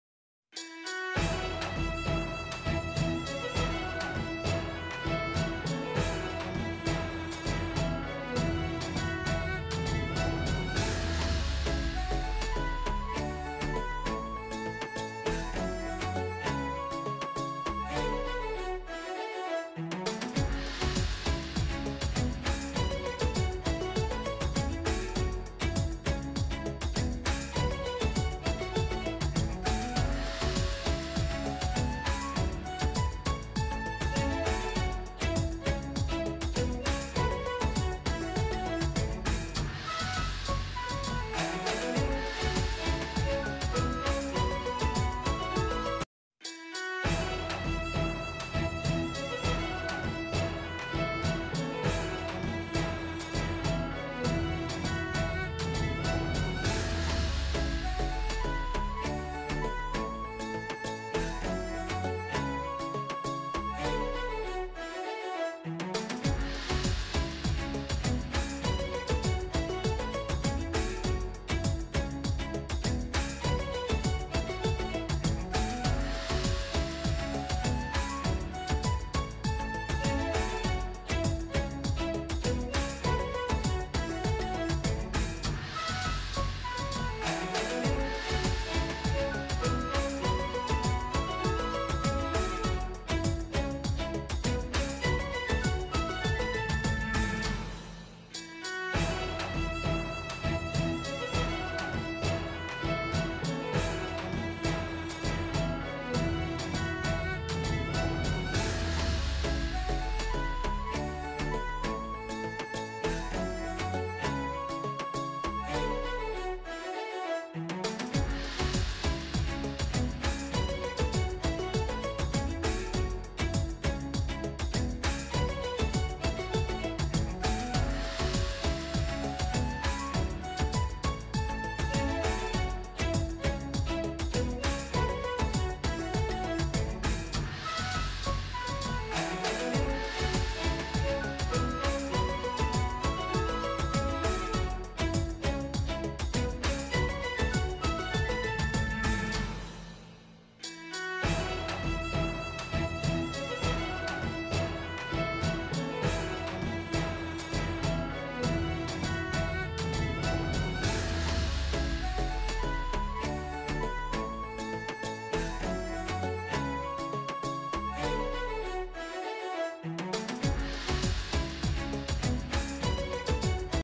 HASIL DAY 3/ BAMTC 2023-PENENTUAN JUARA GROUP C DAN RUNNER UP -INDONESIA VS THAILAND
hasil badminton asia 2023-hasil bulu tangkis hari ini-indonesia vs thailand
B A M T C hari ini#tamplekkanlive#badmintonasia